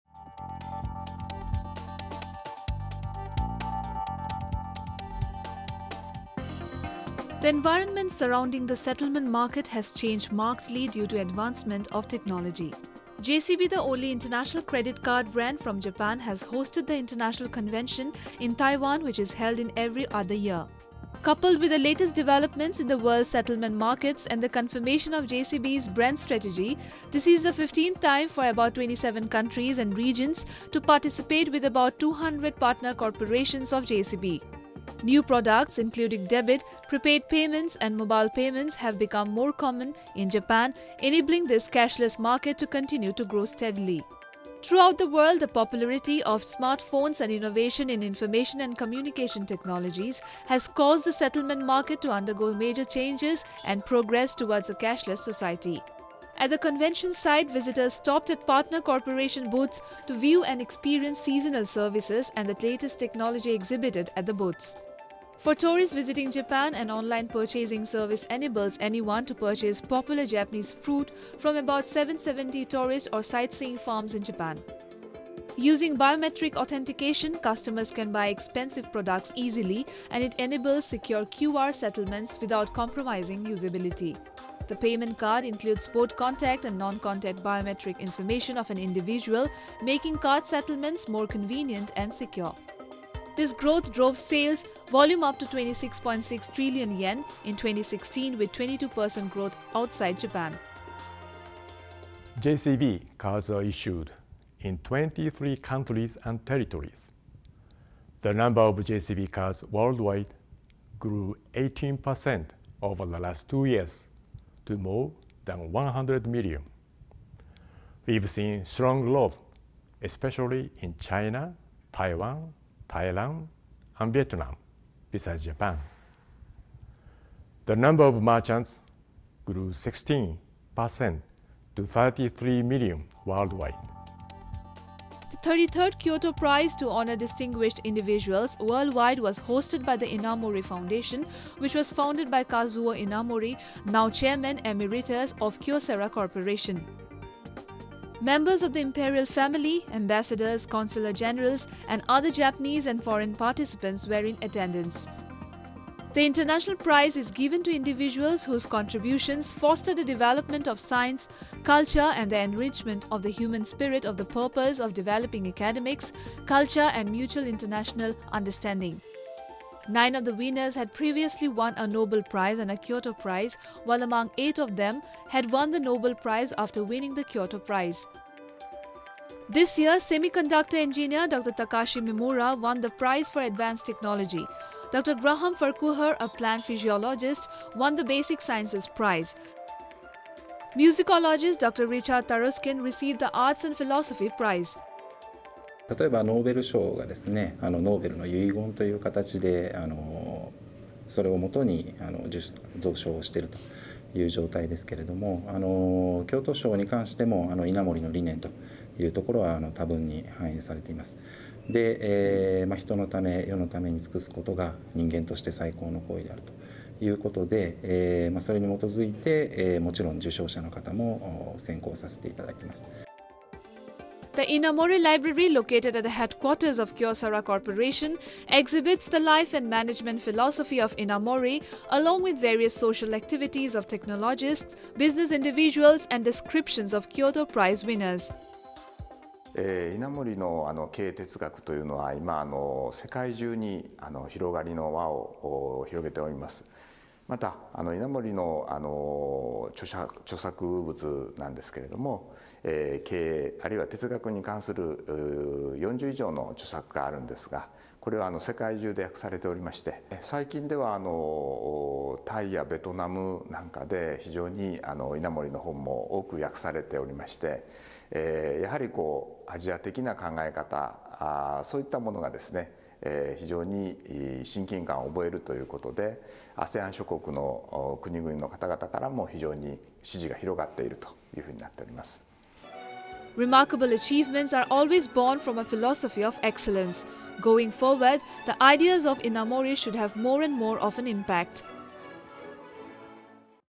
Japanese companies strengthen global ties
Taiwan/ Kyoto, Nov 16 (ANI): The environment surrounding the settlement market has changed markedly due to the advancement of technology.  JCB, the only international credit card brand from Japan, has hosted the international convention in Taiwan which is held in every other year. Throughout the world, the popularity of smartphones and innovation in information and communication technologies has caused the settlement market to undergo major changes and progress towards a cashless society. At the convention site, visitors stopped at partner corporation's booths to view and experience seasonal services and the latest technology exhibited at the booths. The payment card includes both contact and non-contact biometric information of an individual, making card settlements more convenient and secure. This growth drove sales volume up to 26.6 trillion yen in 2016, with 22% growth outside Japan. The 33rd Kyoto Prize to honor distinguished individuals worldwide was hosted by the Inamori Foundation - which was founded by Kazuo Inamori - now Chairman Emeritus of Kyocera Corporation. Members of the Imperial Family, ambassadors, Consul-generals, and other Japanese and foreign participants were in attendance.  Nine of the winners had previously won a Nobel Prize and a Kyoto Prize, while among eight of them, had won the Nobel Prize after winning the Kyoto Prize. The Inamori Library, located at the headquarters of Kyocera Corporation, exhibits the life and management philosophy of Inamori, along with various social activities of technologists, business individuals and descriptions of Kyoto Prize winners.